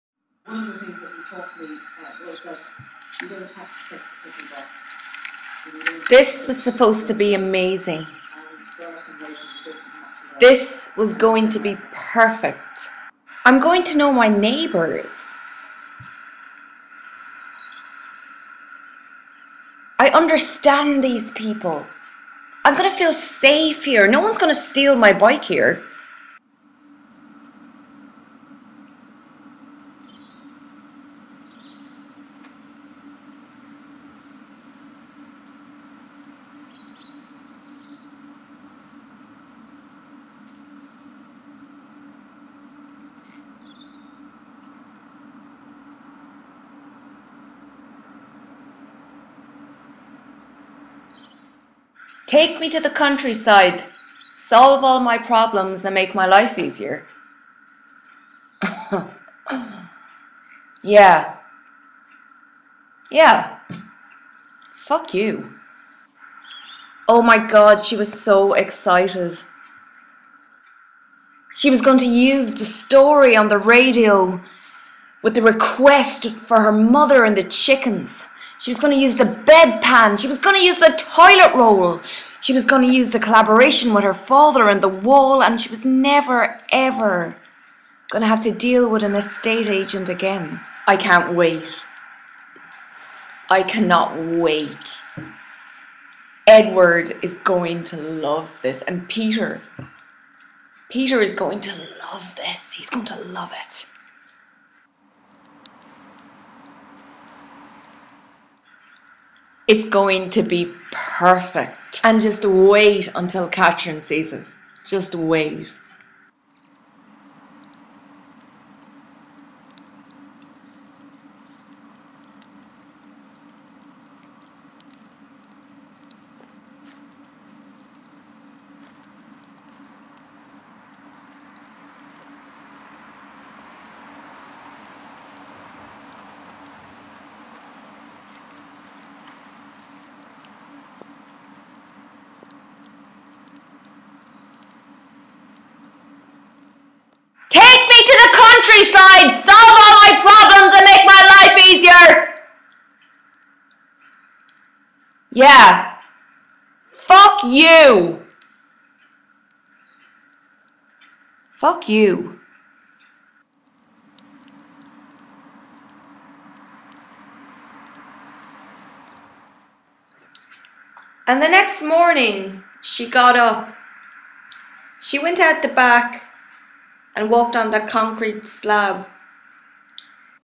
Freelands 2, 2020 (3 min excerpt)
Freelands 2, 2020, was made by invitation to contribute to an instagram artist update post for The Freelands Foundation, UK. The post is the result of a sequential and episodic approach to video making. This work is made in response to my recent switch of positions from living in the city to living in the countryside and is motivated by perceptions of success and failure I have experienced as a professional artist.  The video content explores the extension and continuation of previous artworks I have produced and plays with the trial and error decision processes and intuitiveness of making and doing. Thematically, the work deals with the engagement of factual and fictional narratives surrounding the lives of professional artists based upon the influences of their immediate environment.